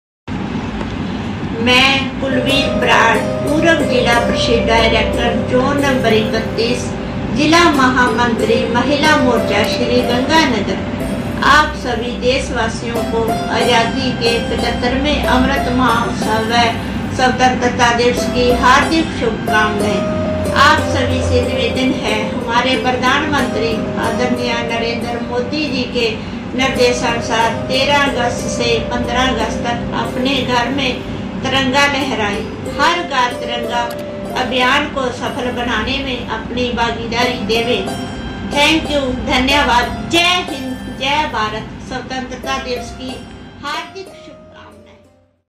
केसरीसिंहपुर भाजपा नेत्री कुलवीर कौर बराड़ ने स्वतंत्रता दिवस की दी शुभकामनायें
केसरीसिंहपुर कस्बे के सीमावर्ती गावं जोधेवाला निवासी व पूर्व जिला परिषद डायरेक्ट व भारतीय जनता पार्टी के महिला मोर्चा से  जिला महामंत्री व भाजपा नेता अमृत पाल सिँह जी की धर्मपत्नी श्री मति कुलवीर कौर ने आजादी के 75 वे अमृत महोत्सव व स्वतंत्रता दिवस की सभी देशवासियो को दी शुभकामनायें